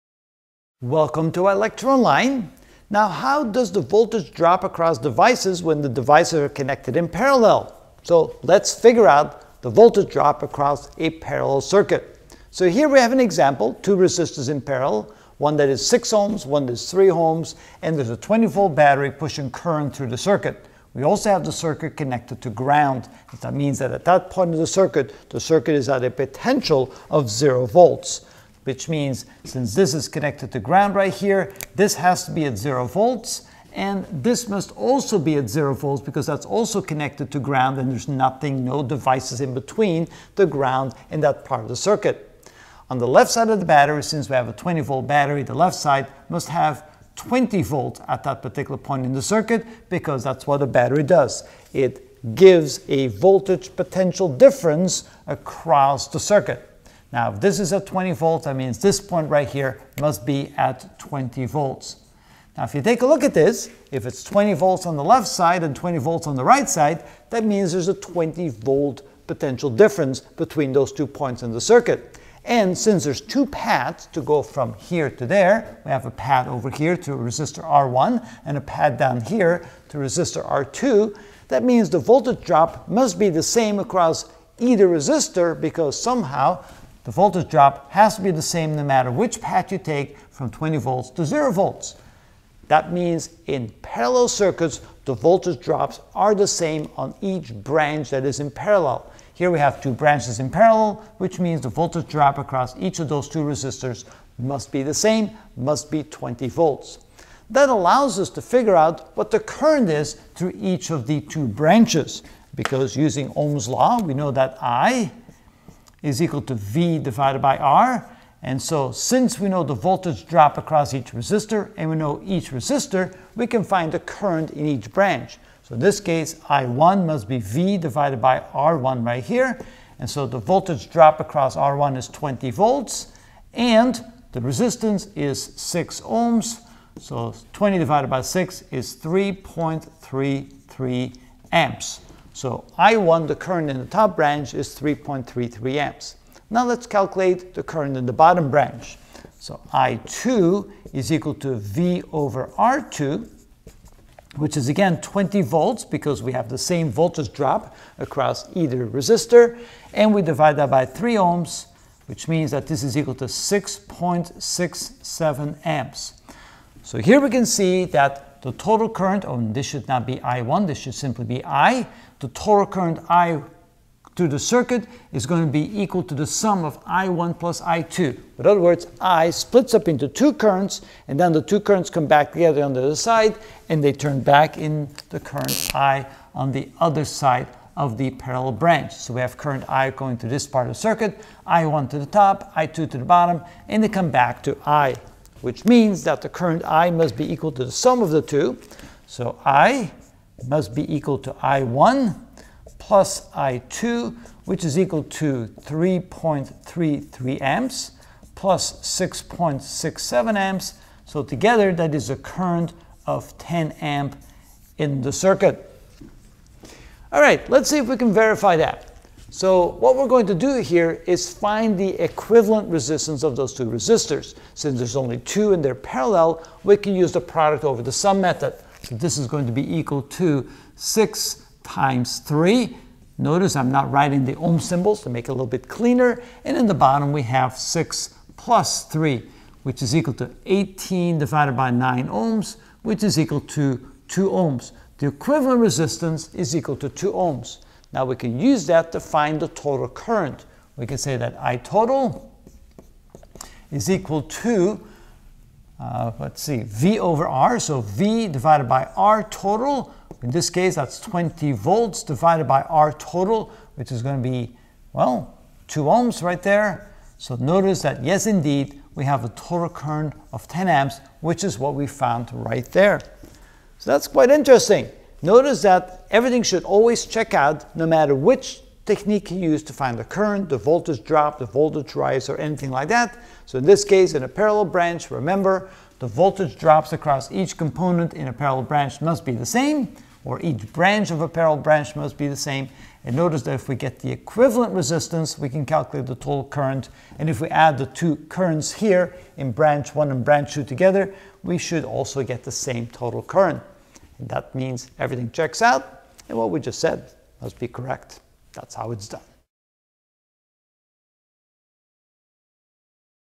Physics - E&M: Ch 41.3 Ohm's Law & Resistor Circuit Understood (6 of 27) Voltage Drop in // Circuit
In this video I will calculate the voltage drop across a parallel circuit with a 20V battery pushing a current through the circuit. The circuit is grounded which means the voltage there is 0V. The equations needed are R(total)=(R1R2)/(R1+R2)? and I=V/R=?